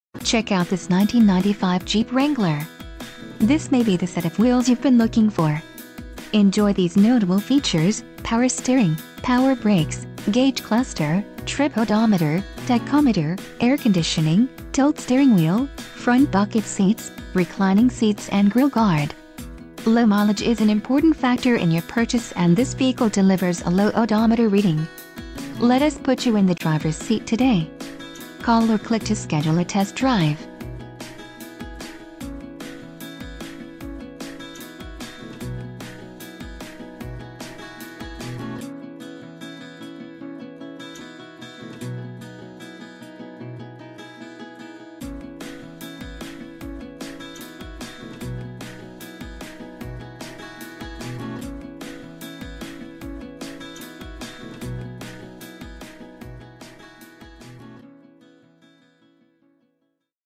1995 Jeep Wrangler - Cordele, GA - Sunbelt Ford, GA 31015
For Sale in Sunbelt Ford, GA 31015

1995 Jeep Wrangler Condition: Used Mileage: 175580 Engine: 6 Transmission: Manual Exterior Color: Black Interior Color: Black / Gray Doors: 2 Stock